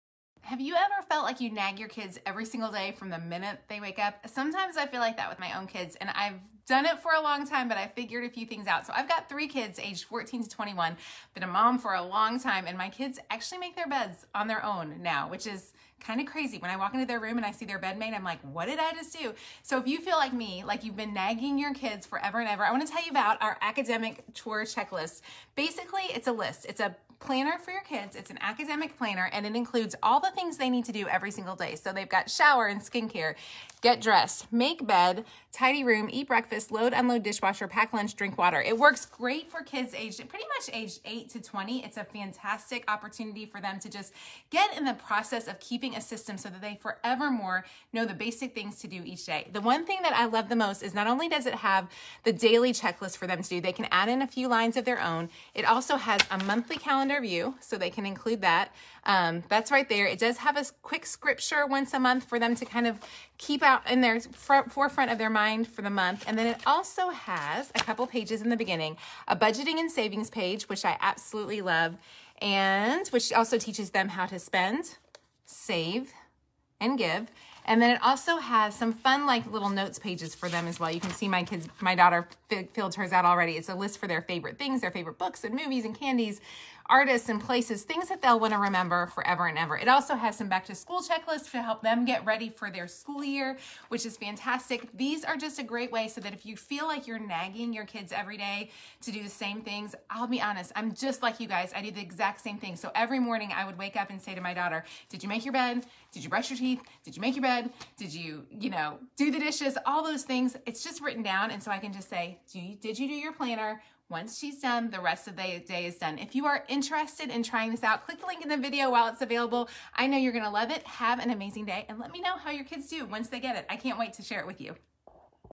Kids Academic Planner
Kids planner ~ physical product and PDF version available now!

Students will get organized, stay focused, and be inspired to make this school year the best year ever with the new Academic Planner.

Journal and doodle all over the vision board and favorite things PAGES. Plan ahead for things like back to school, birthday parties, Christmas, and fun trips! When they need money to plan all those fun things, the budget and savings worksheets can help create financial goals and know just what needs to be done to save, spend and give!

Of course, a planner for academics will include some of those pages as well. There is a class schedule layout to add details for school schedules and goals for the semester. The notes pages are lined for notes, to-do lists, or personal checklists.

The monthly sections include a 2-page calendar layout for the big picture planning along with a monthly scripture. Finally, the weekly planner pages include room to add due dates and details (maybe some fun??) and have the popular PPP checklists with tasks that are geared toward students.

FOR MORE INFO ON printable & online coupons for retailers like Amazon, Walmart, Target & more.